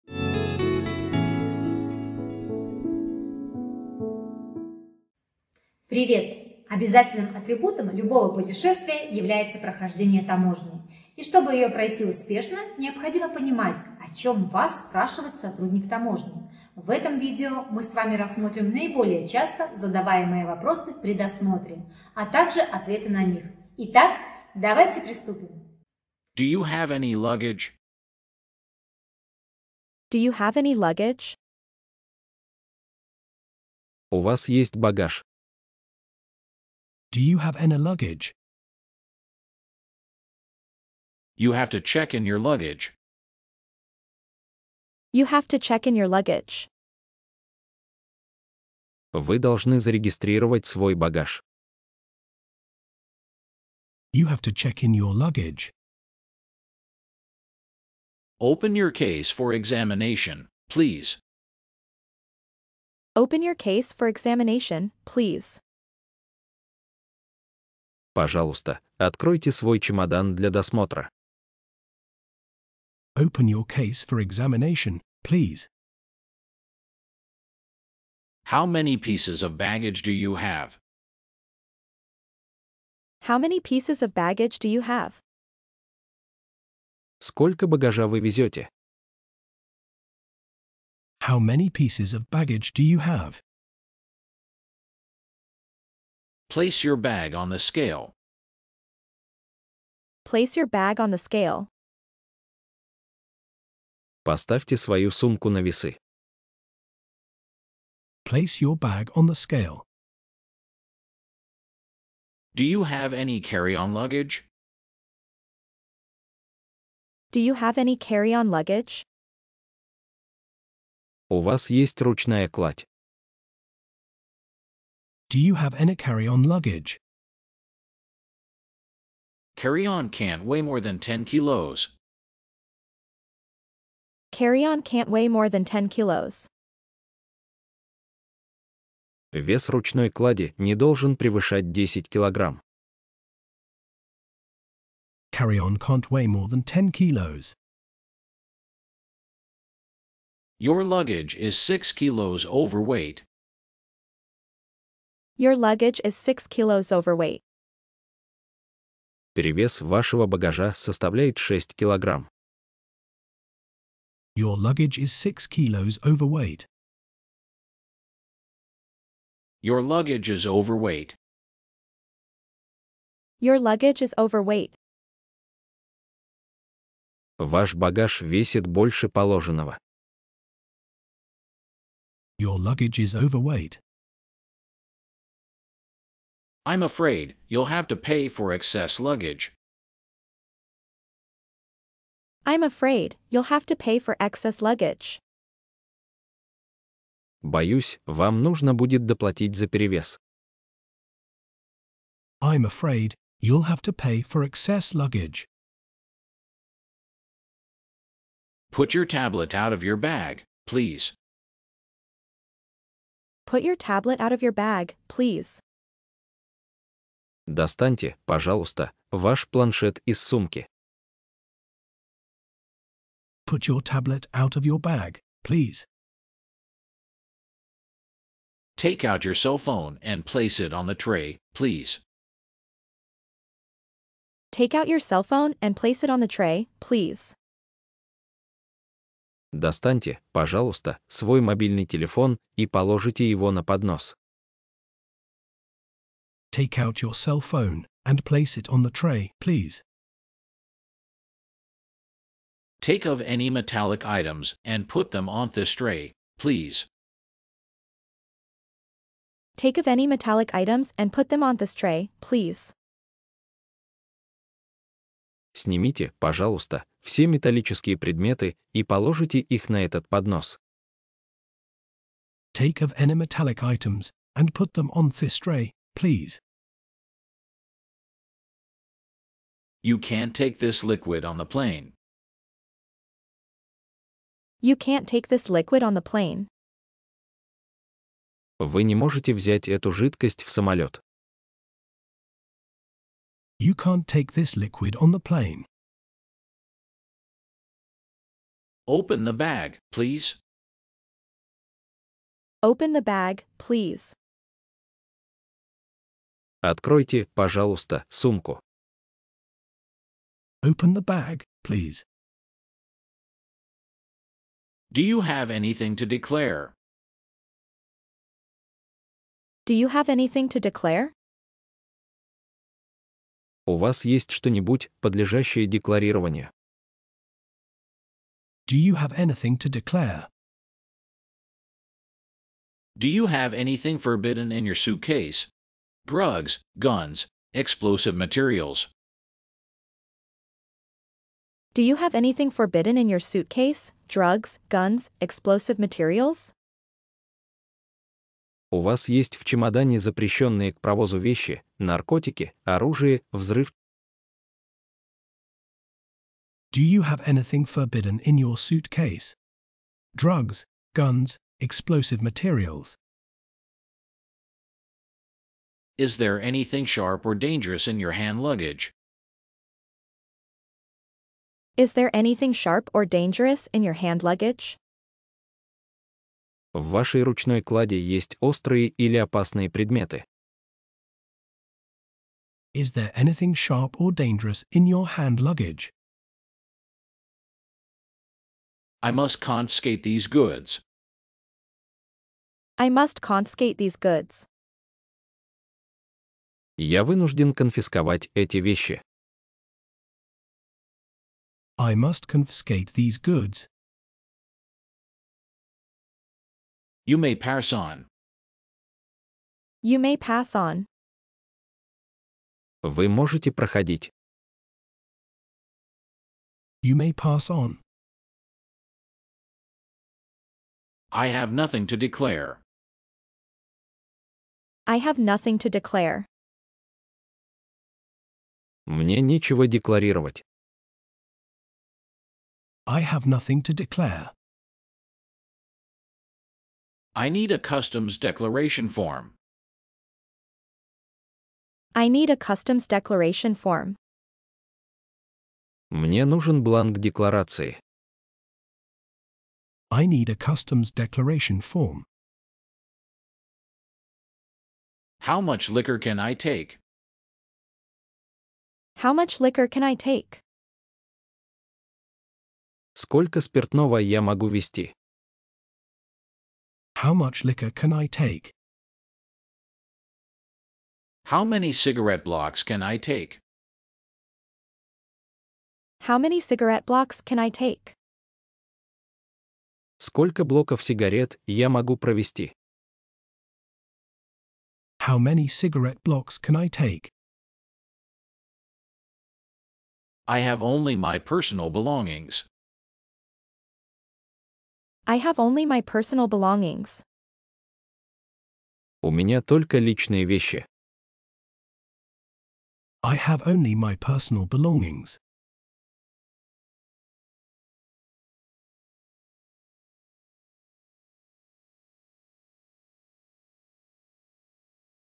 Фразы и предложения для прохождения таможни. Английский язык. English.
В этом видео каждая фраза или предложение произносятся несколько раз мужским и женским голосом с американским и английским акцентом.  На экране появляются субтитры с их правильным написанием. Кроме этого вы услышите перевод этих слов и фраз на русский язык.

Наши другие полезные видео для изучения английского:
Английский по плейлистам